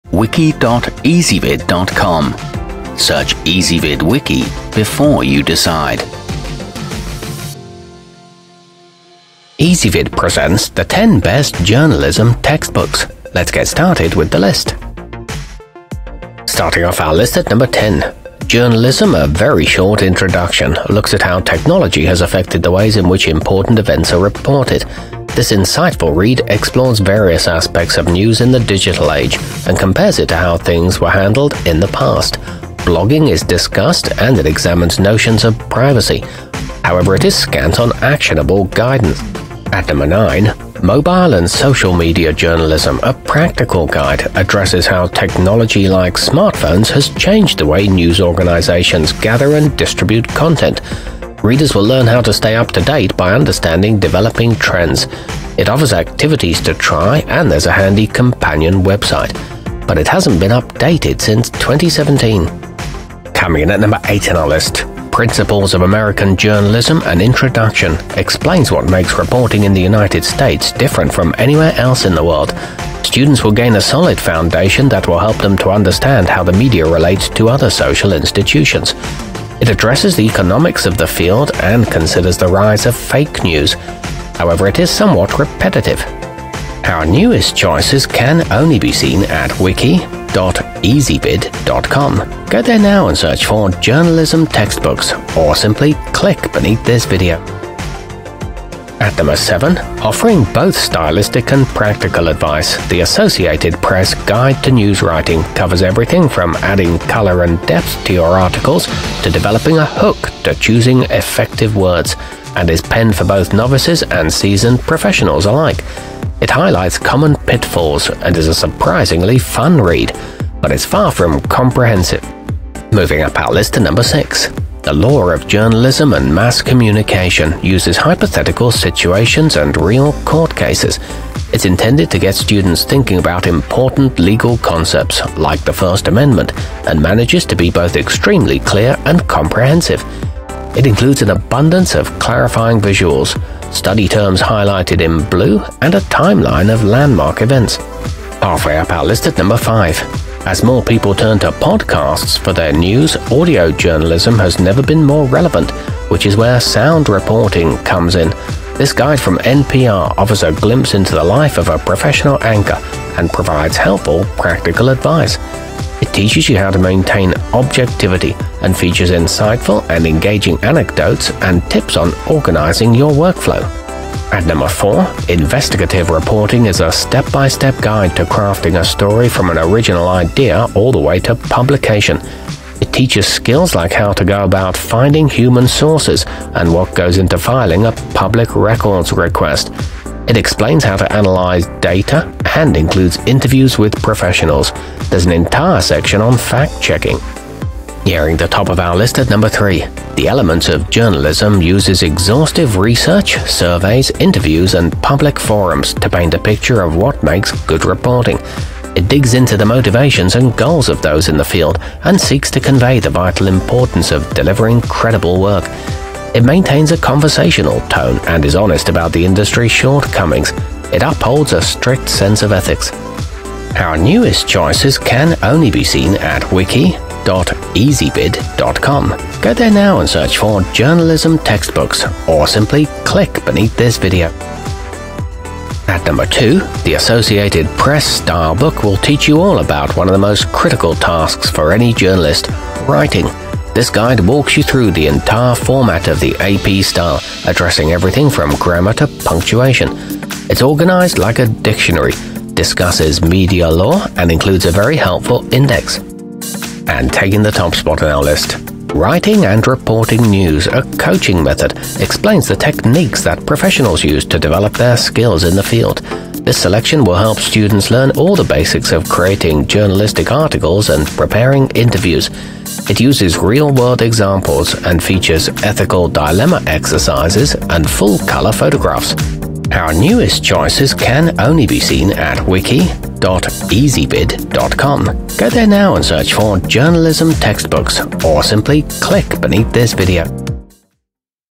10 Best Journalism Textbooks 2020
Our complete review, including our selection for the year's best journalism textbook, is exclusively available on Ezvid Wiki.

Journalism textbooks included in this wiki include the associated press guide to news writing, principles of american journalism: an introduction, the elements of journalism, the associated press stylebook, writing and reporting news: a coaching method, investigative reporting, the law of journalism and mass communication, mobile and social media journalism: a practical guide, journalism: a very short introduction, and sound reporting.